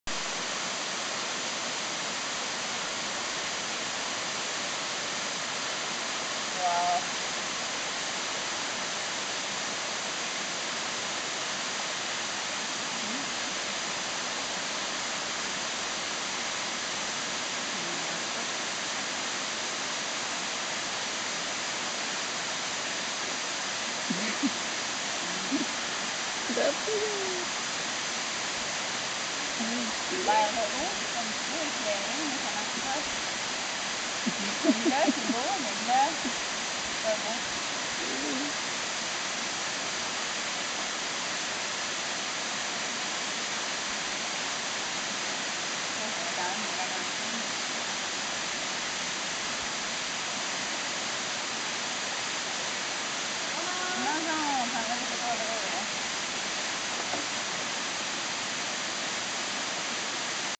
Inchworm!
A very small inchworm in La Tigra National Park in Honduras